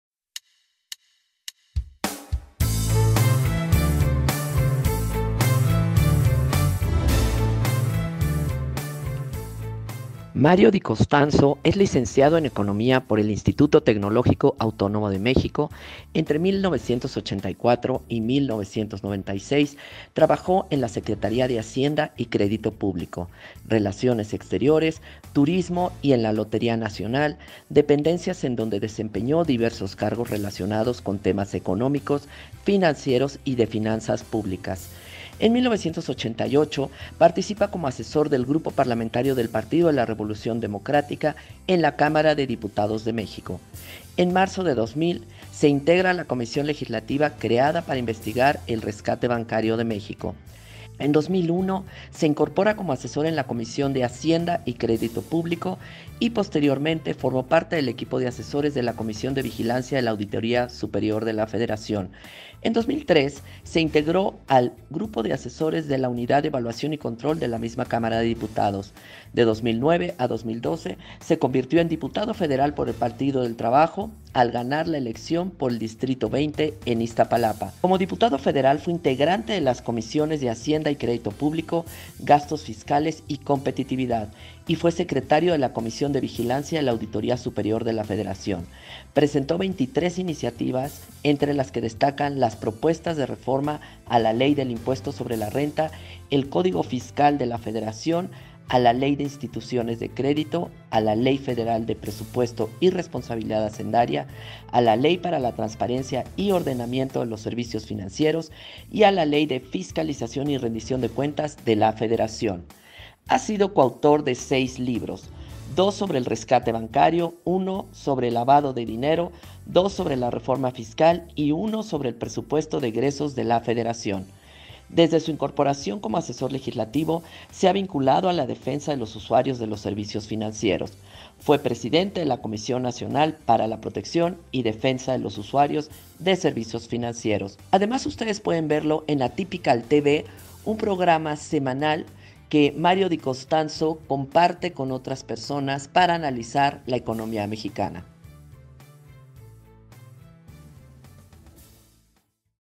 Conoce la trayectoria de Mario Di Costanzo
Mario Di Costanzo es Economista. Analista financiero.
 Ex presidente de Condusef.